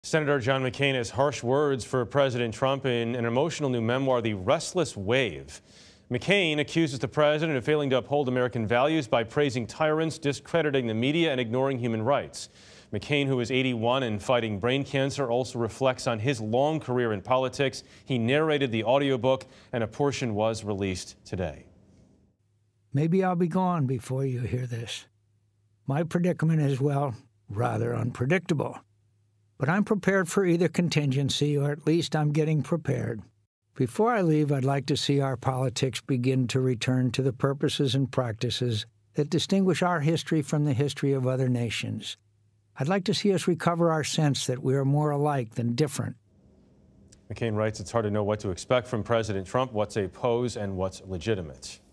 Emotional portion of McCain audiobook released
Senator John McCain, who is fighting brain cancer, is reflecting on his long career in a new memoir. He narrated the audiobook, and a portion of it was released on Thursday.